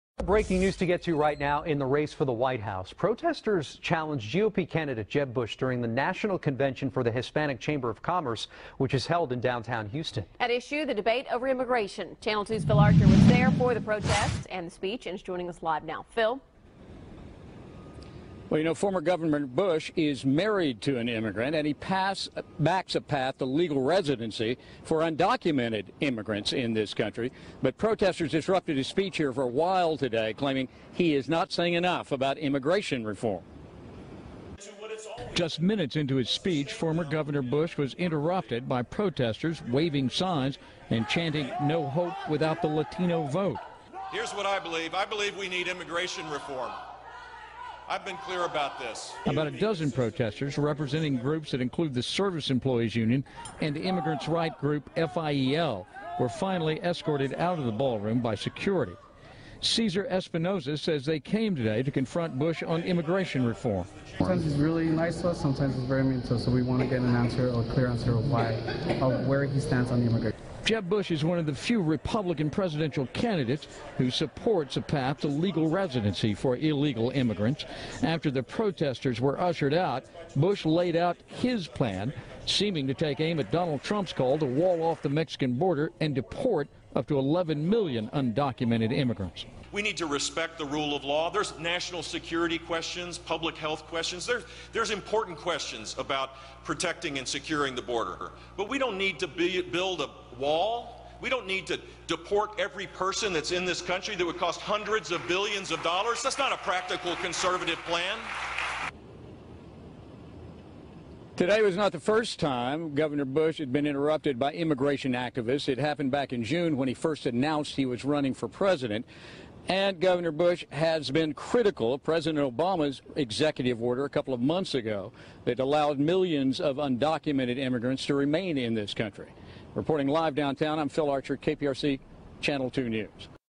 15-0922 Jeb! Gets Heckled by Pro-Amnesty Group
Jeb! gets heckled by pro-amnesty groups because he's not far enough left.